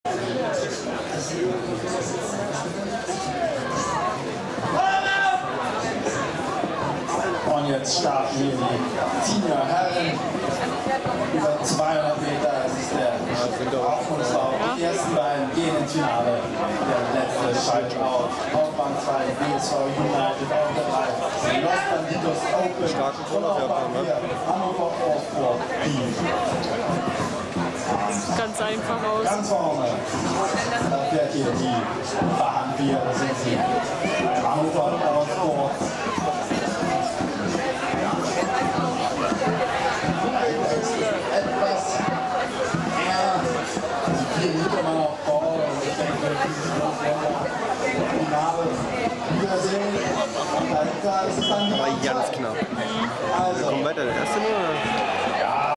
8. Deutsche Drachenboot Meisterschaft 200m Herren
Hoffnungslauf bei der 8. Deutschen Drachenboot Meisterschaft über 200m Herren

Ergebnis:
Hannover All Sports Team         00:47,58
WSV United                               00:47,68
Los Banditos Open                    00:48,33